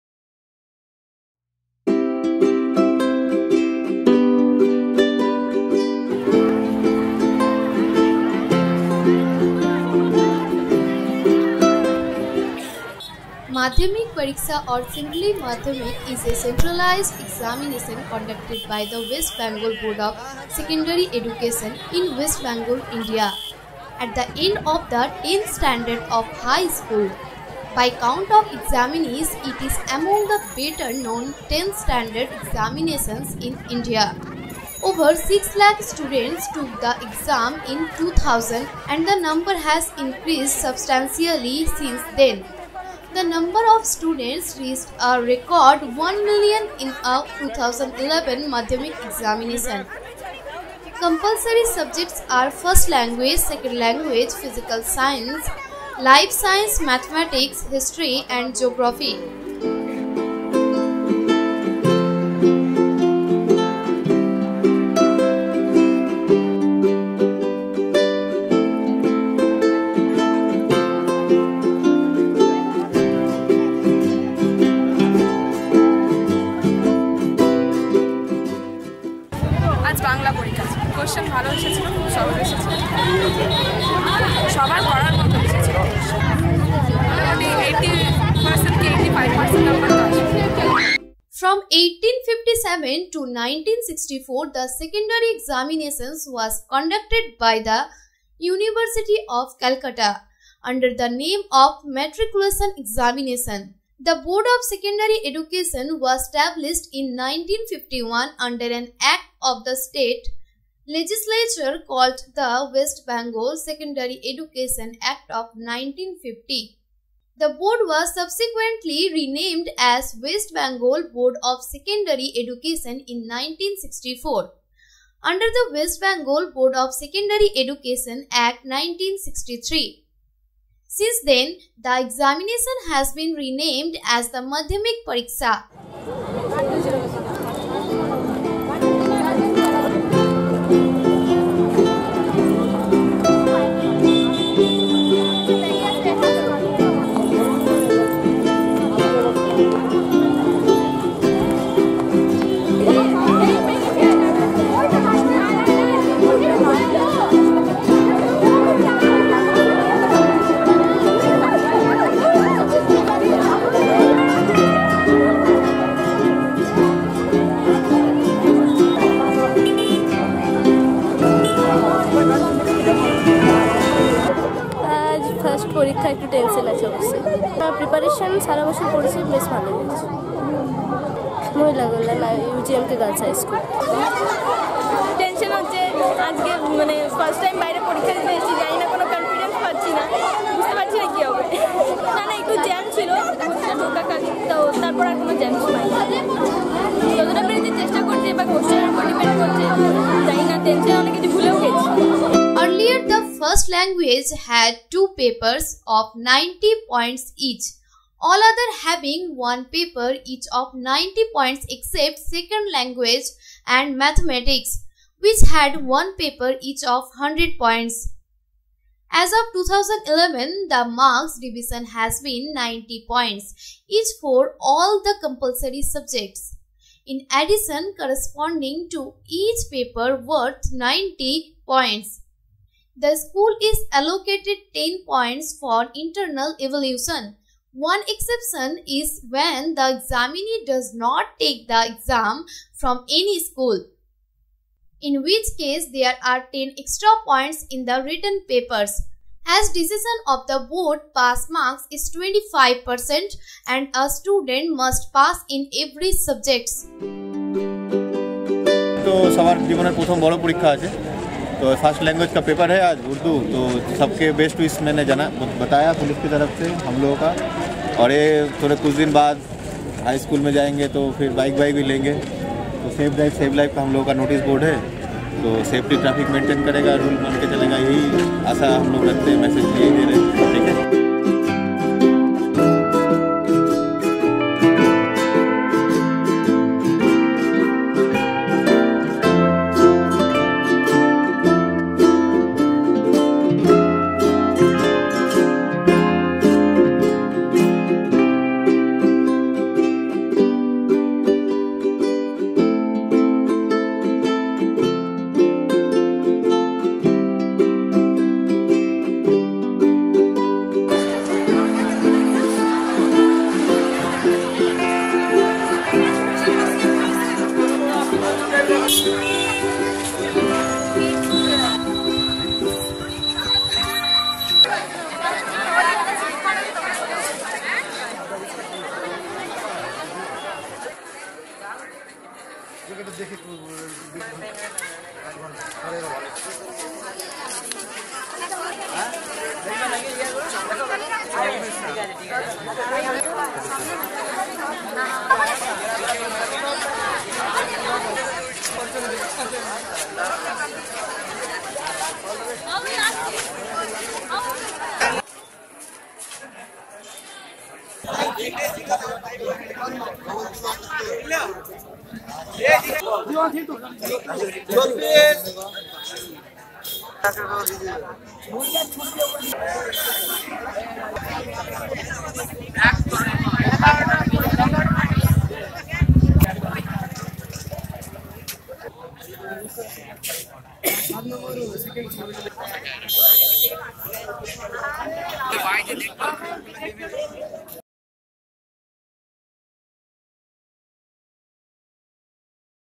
all about : Madhyamik Pariksha।।RANIGANJ।।ASANSOL।।DURGAPUR।।BURDWAN।।WEST BENGAL INDIA
Madhyamik Pariksha
Madhyamik Pariksha or simply Madhyamik (Bengali: মাধ্যমিক) is a centralised examination conducted by the West Bengal Board of Secondary Education (পশ্চিমবঙ্গ মধ্যশিক্ষা পর্ষদ) in West Bengal, India, at the end of the 10th standard of high school. By count of examinees, it is among the better known 10th standard examinations in India. Over 600,000 students took the exam in 2000, and the number has increased substantially since then. The number of students reached a record 1 million in the 2011
Madhyamik examination. Compulsory subjects are first language, second language, physical science (combined with physics and chemistry), life science, mathematics, history and geography
HISTORY
From 1857 to 1964, the secondary examination was conducted by the University of Calcutta, under the name of matriculation examination. The Board of Secondary Education was established in 1951 under an Act of the State Legislature called the West Bengal Secondary Education Act of 1950. The board was subsequently renamed as West Bengal Board of Secondary Education in 1964, under the West Bengal Board of Secondary Education Act 1963. Since then, the examination has been renamed as the Madhyamik Pariksha (literally secondary examination in Bengali; the word madhyamik translates from the Bengali language as middle level)

Earlier, the first language had two papers of 90 points each, all others having one paper each of 90 points except Second Language and Mathematics, which had one paper each of 100 points.

As of 2011, the marks division has been 90 points each for all the compulsory subjects. In addition, corresponding to each paper worth 90 (rather than the full 100) points, the school is allocated 10 points for internal evaluation. One exception is when the examinee does not take the exam from any school, in which case there are 10 extra points in the written papers. As, decision of the board pass marks is 25%. And a student must pass in every subjects